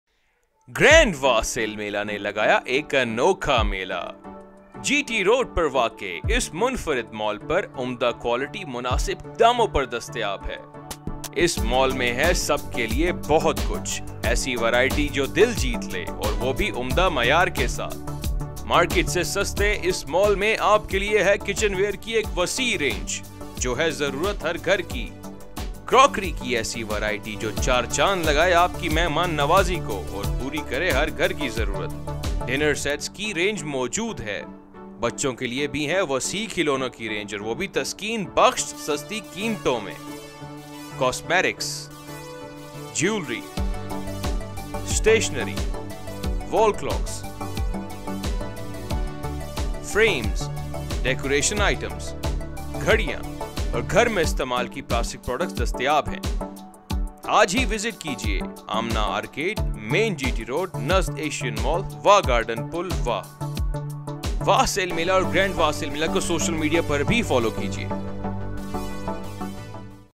GRAND WSM (GRAND WAH SALE MELA) GT road Wah Garden
Grand WSM is a shopping mall where there is something for everyone. From Toys, Mobile Accessories, Household items, Kitchen Accessories, Gift items and so much more that you'll have to visit to see. Easily accessible on main G.T. Road near Wah Gardens, Grand WSM is located in the Ground Floor of Amna Arcade. However, that's not all! while Grand WSM outlet is within easy reach of surrounding cities, we will also have an online store which you can visit anytime from anywhere and order your favourite products from the comfort of your homes. We have built a shopping destination where the whole family will be excited to visit.